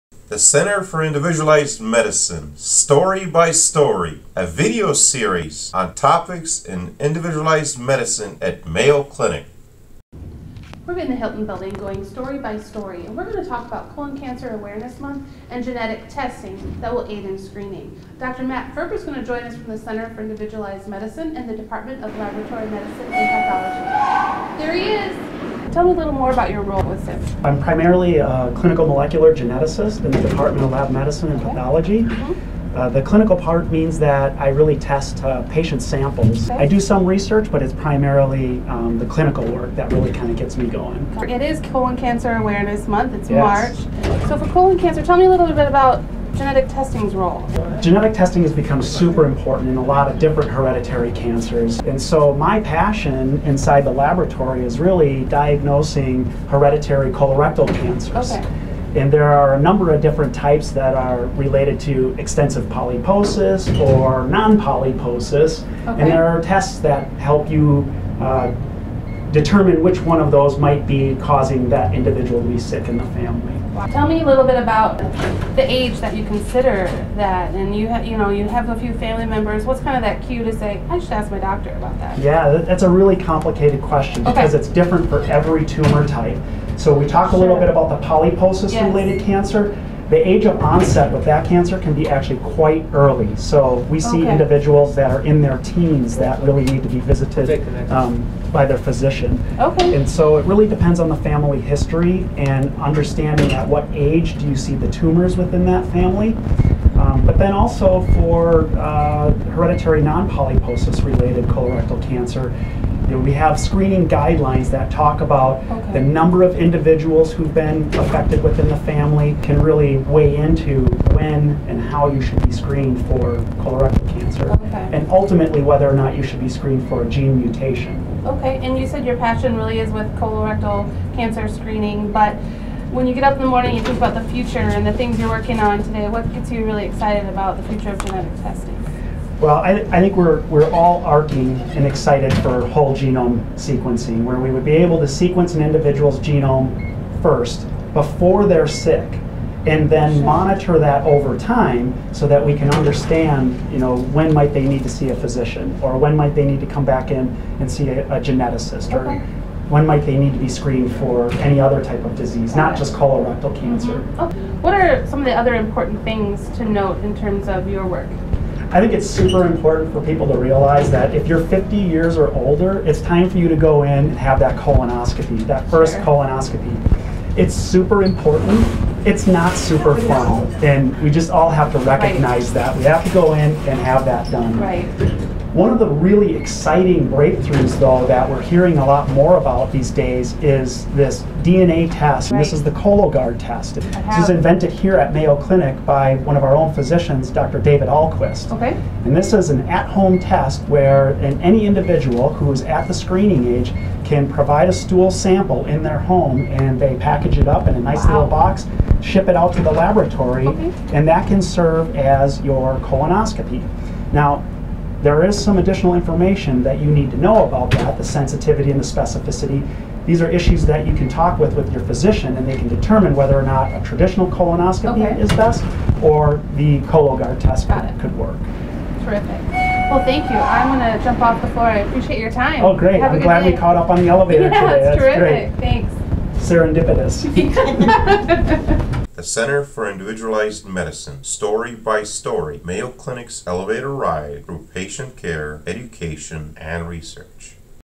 Story by Story Episode 14: Colorectal Cancer Genetic Screening and Awareness
In Story by Story Episode 14, Dr. Matthew Ferber discusses his role with the Center for Individualized Medicine and  genetic testing related to colorectal cancer awareness.

For more information on precision medicine topics, clinical genetics and the Center for Individualized Medicine, join us for the Individualizing Medicine 2017 conference, October 9-10, 2017.